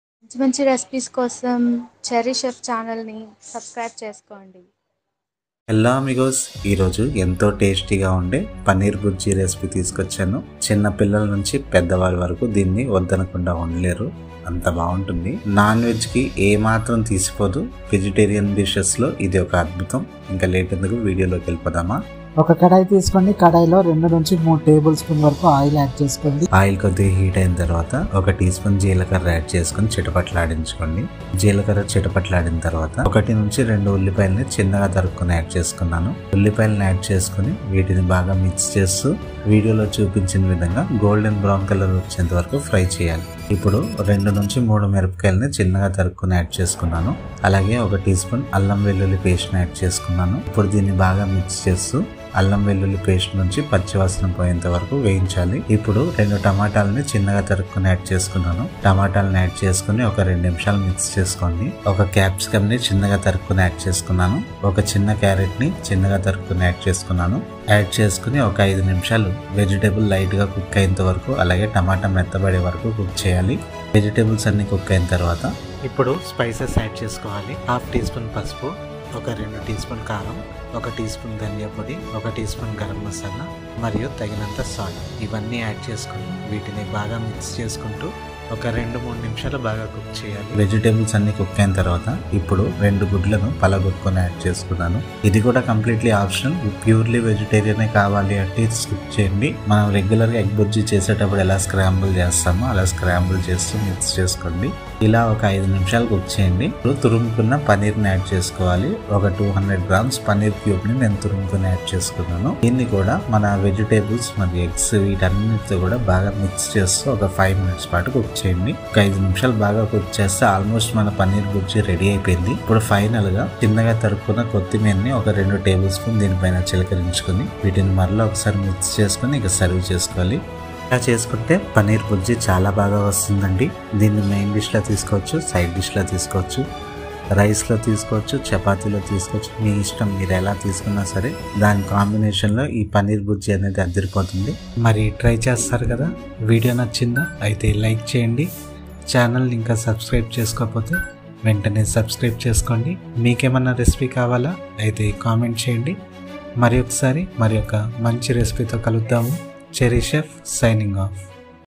Cherry Chef's Quick & Easy Paneer Bhurji Recipe
Welcome to Cherry Chef's kitchen! Today, we're making a quick and easy Paneer Bhurji that’s perfect for breakfast, lunch, or dinner. This delicious Indian scrambled paneer dish is packed with flavors and can be whipped up in no time. Whether you're a seasoned chef or a beginner, this recipe is simple to follow and will leave you wanting more.

Ingredients:
1. Oil 2-3Tbsp
2. Cumin Seeds 1Tsp
3. Chopped Onion 1-2pc
4. Chopped Green Chillies 2-3pc
5. Ginger-Garlic Paste 1Tsp
6. Chopped Tomato x2
7. Chopped Bell Pepper x1 (Optional)
8. Chopped Carrot x1(Small) (Optional)
9. Eggs x2 (Optional)
10. Turmeric Powder Half Tsp
11. Red Chilly Powder 2Tsp
12. Coriander Powder 1Tsp
13. Garam Masala 1Tsp
14. Grated Paneer 200g
15. Salt (Adjustable)
16. Cilantro 2Tbsp (Garnish)